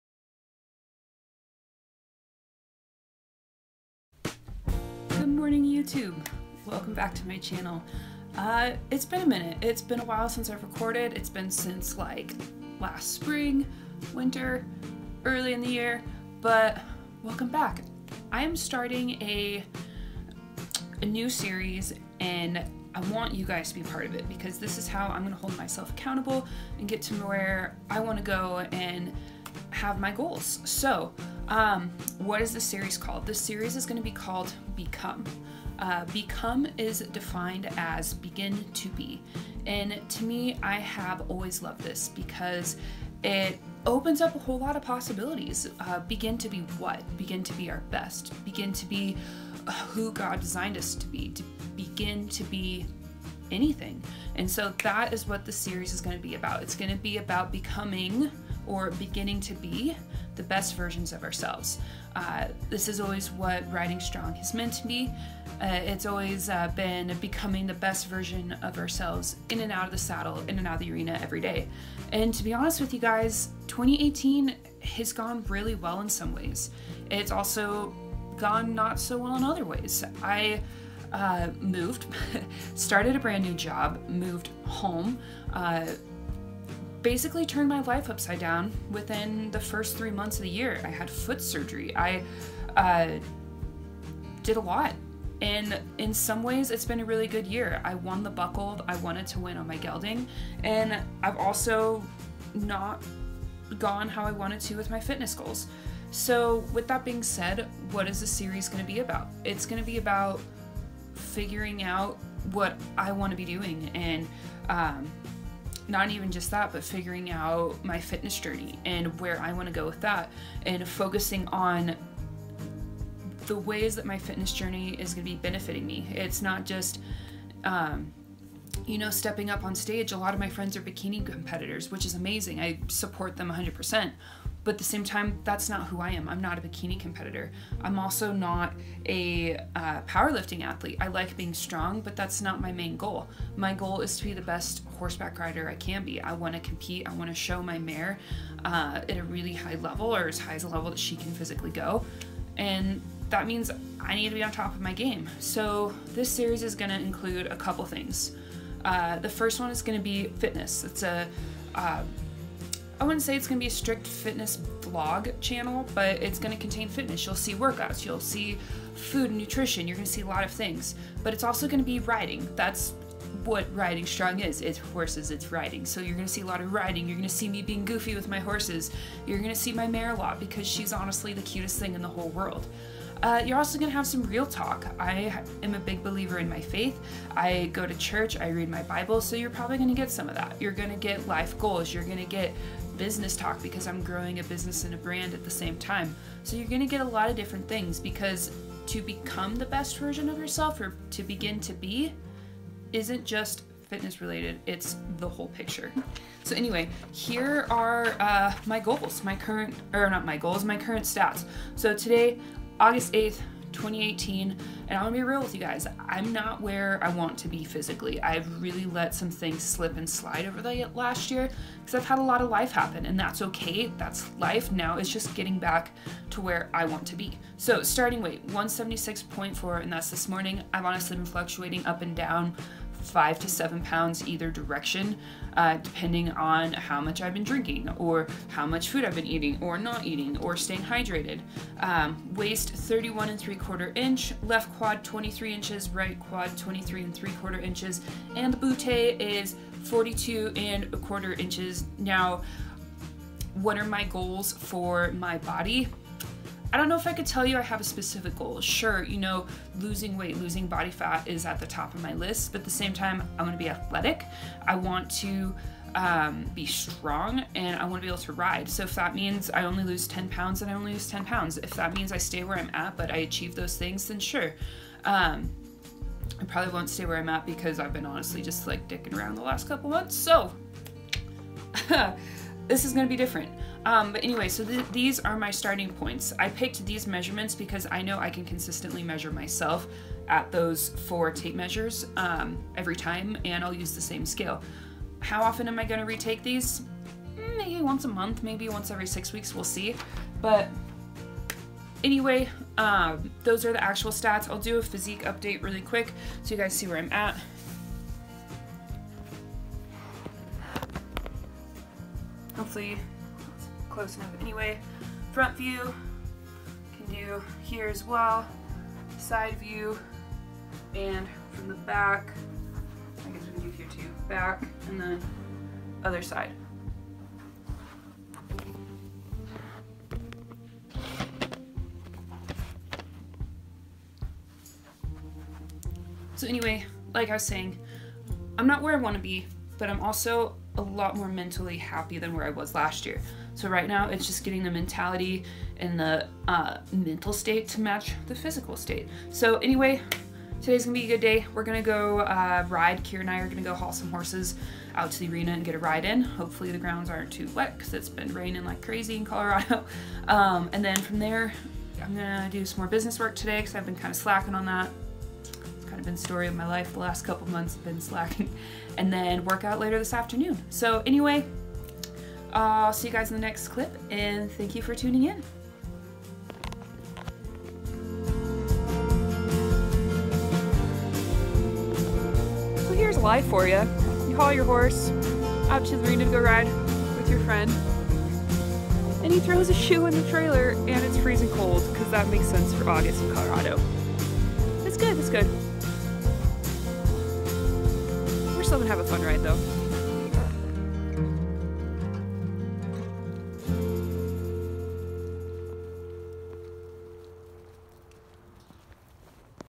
Become - Ep. 1
Hey y'all! Welcome to Episode 1 of my new series called Become.

This series will take us all through my journey of rediscovering health and fitness, but as it relates to riding horses! When I first fell in love with health and fitness, it was right when competing in bodybuilding became popular and trendy (about 4 years ago). So naturally I jumped on the band wagon, or tried to, and honestly it wasn't for me. I tried powerlifting next and realized that interfered with my one true love; riding horses.

Fast forward 4 years and here I am, owning a fitness for horseback riders business called Riding Strong, and I have lost my own fitness. It's life, it happens, and I encourage you all to tag along with me on my journey of becoming the best version of myself (cliche I know but I like it).

If you're still reading this, you are a doll!! Hit a thumbs up for me and share/comment till your heart's content!